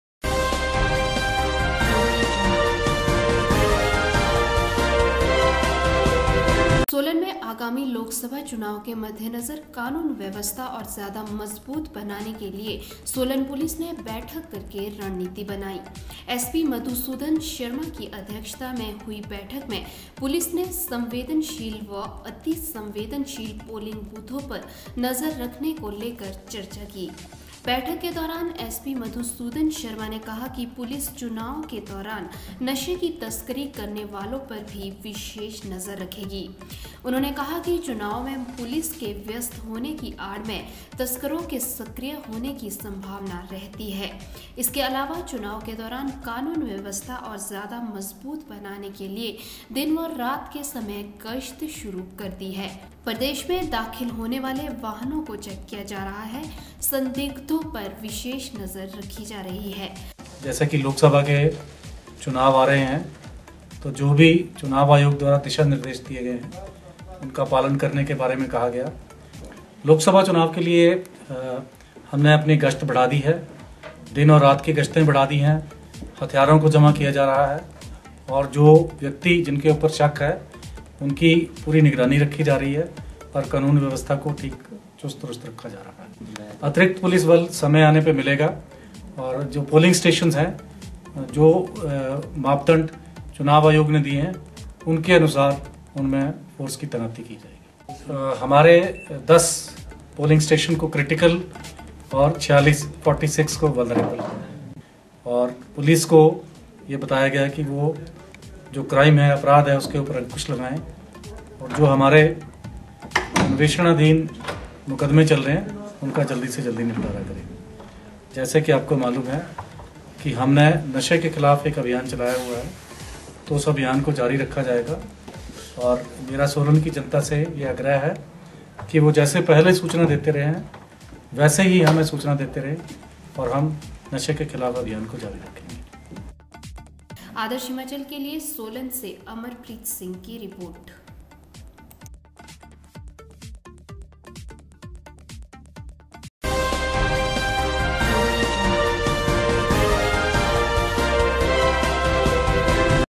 लोकसभा चुनावों के लिए कानून व्यवस्था मजबूत बनाने के लिए सोलन पुलिस ने बनाई रणनीति
आगामी लोकसभा चुनावों के लिए कानून व्यवस्था मजबूत बनाने के लिए सोलन पुलिस ने बनाई रणनीति...एसपी मधुसूदन शर्मा की अध्यक्षता में हुई बैठक. संवेदनशील व अतिसंवेदनशील पोलिंग बूथों पर नजर रखने को लेकर पुलिस ने की चर्चा. एसपी मधुसूदन शर्मा ने कहा .चुनावों के दौरान नशे की तस्करी करने वालों पर भी विशेष नजर रखेगी पुलिस